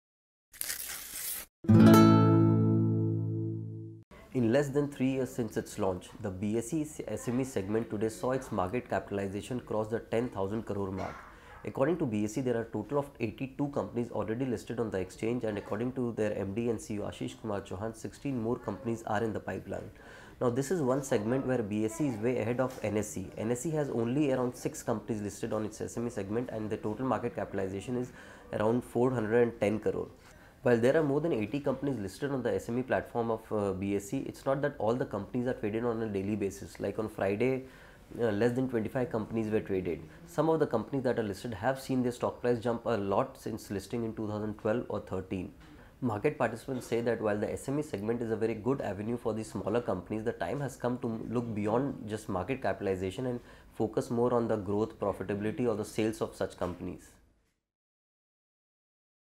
Market capitalization of SMEs tops `10,000 crore on BSE
Nearly three years after the BSE launched a separate platform for small and medium enterprises (SMEs), the market capitalization of the segment crossed `10,000 crore for the first time on Friday.